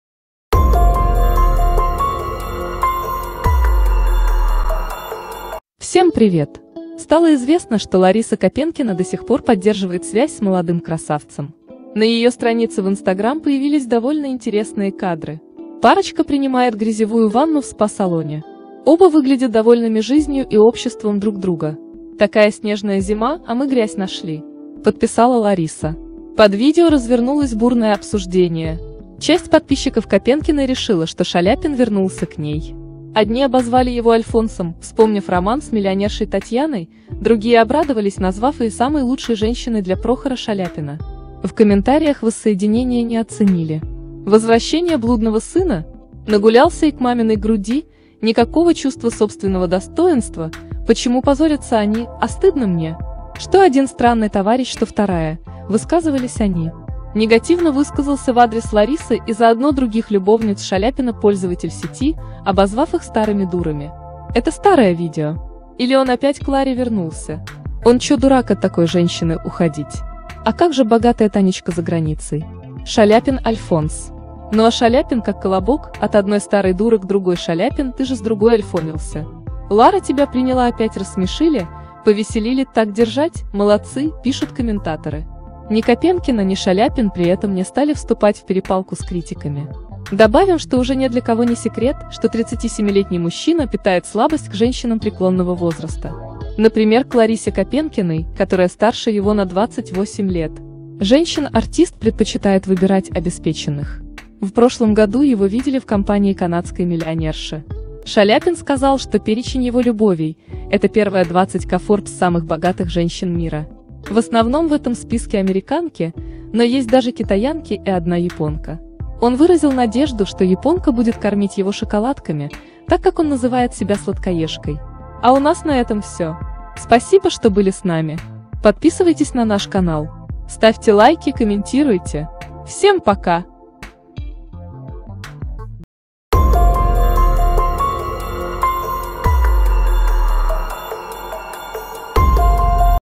Упавший лицом в грязь Шаляпин вернулся к бывшей!
Упавший в грязь Шаляпин наступил на те же грабли.